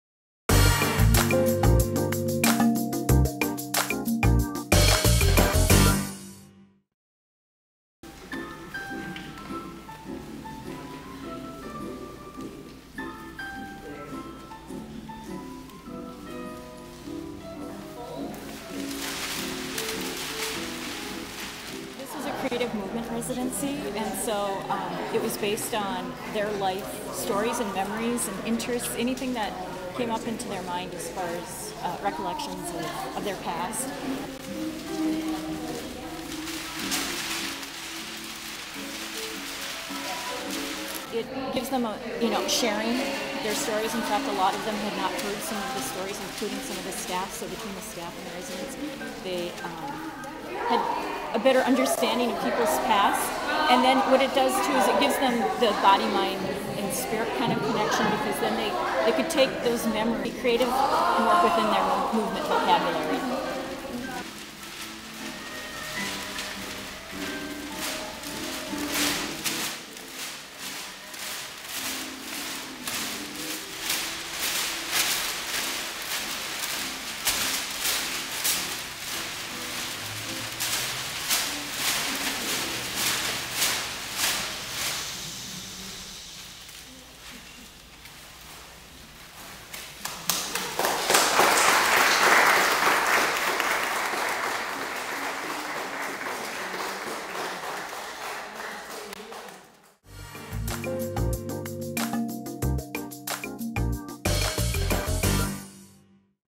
residentartist.mp4
St. Gertrude's Health and Rehabilitation Center staff and residents participated in a Residency in Arts program Friday. Staff member Tracie Weathers, resident Gisella Mieseler, resident Adelaide Prillaman and staff member Monica Perkins pay "Homage to the Library" in creative movement with artist Ann-Marie Lousine-Thomsen.